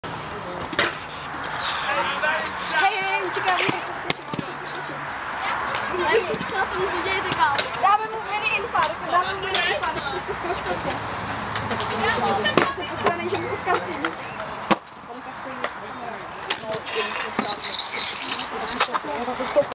Inline Skating - Parallel and Crossover Turns, Jumps Up, Diagonal Mute Grab Drop
USD Aeon Nick Lomax Pro 60

Powerslide Crash Pad Short Standard